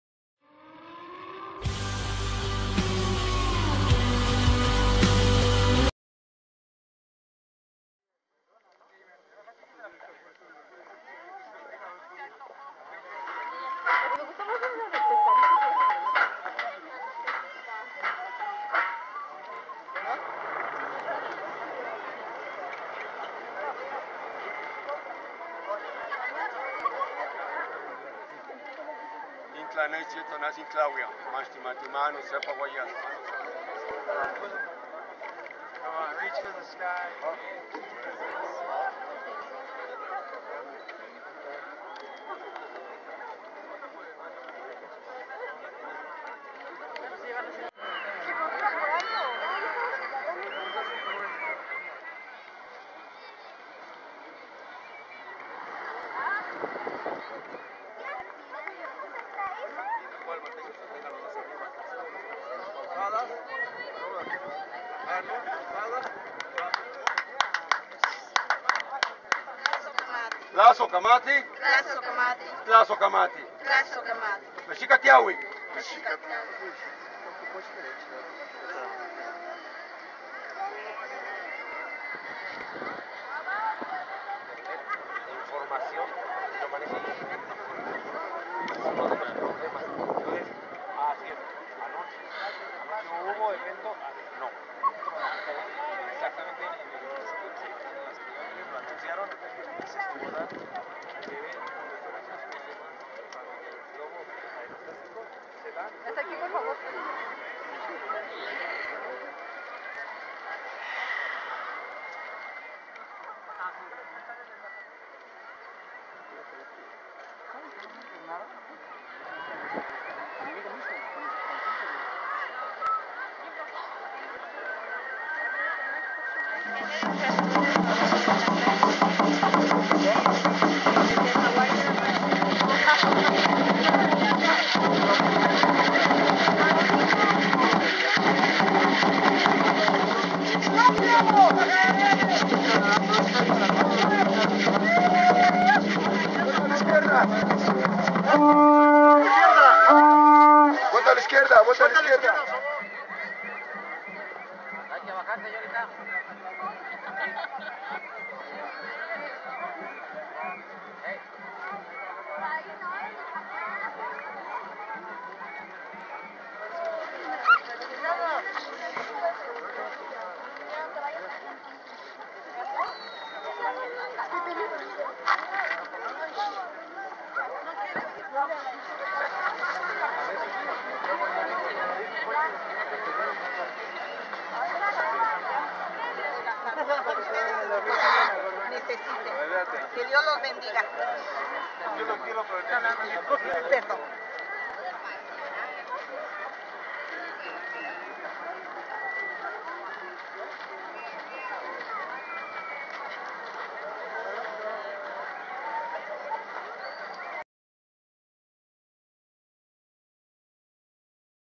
Teotihuacán en el inicio de la Primavera
Agencia MANL

Teotihuacán, Méx.- Más de 250 mil personas acudieron el fin de semana y el 21 de marzo a la zona arqueológica a cargarse de energia.